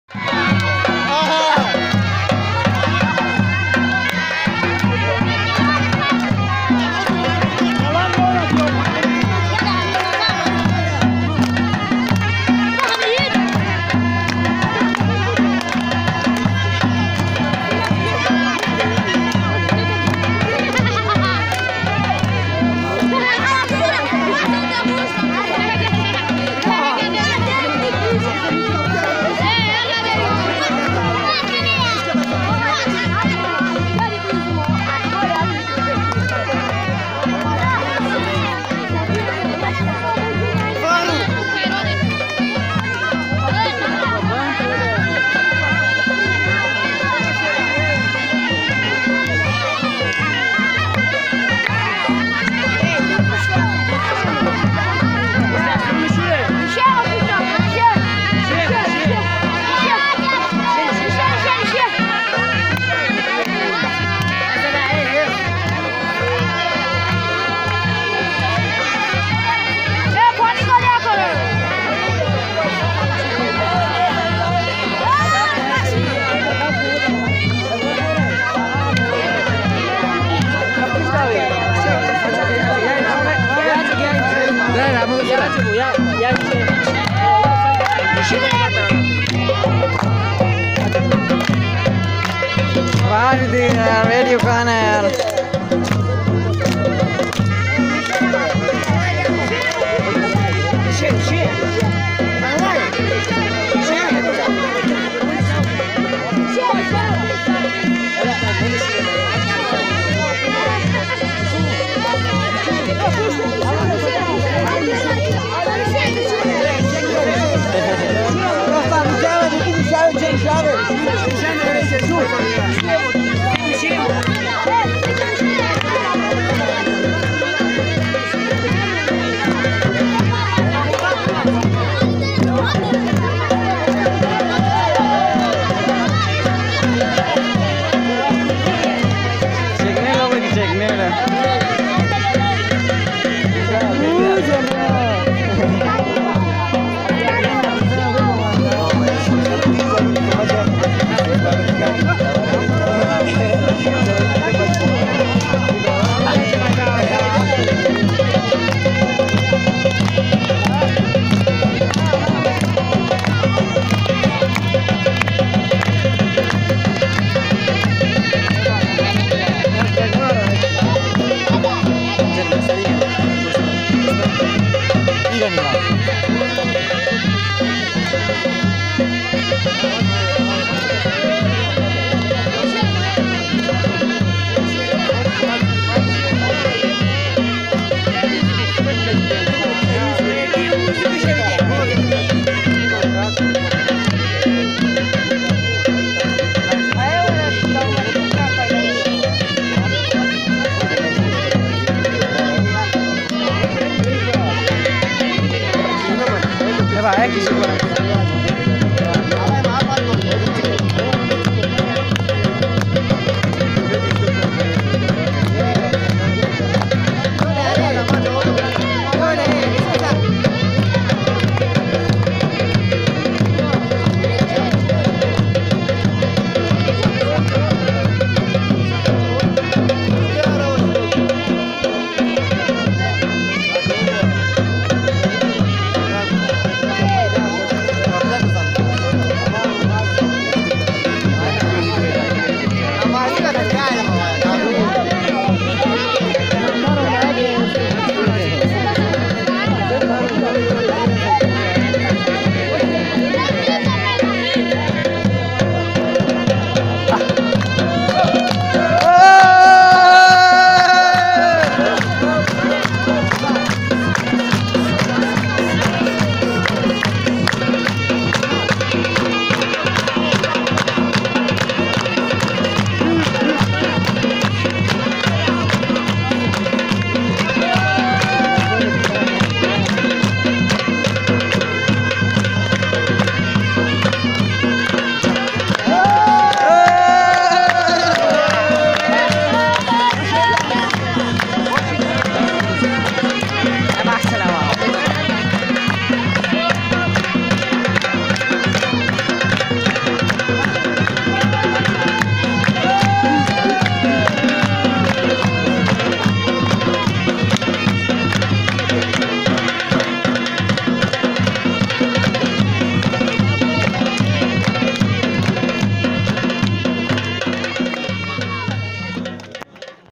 Tanveer Ahmad Dance in Brep Chitral 2019
Tanveer Dance in Brep Chitral 2019 with reshun dhol ishtok.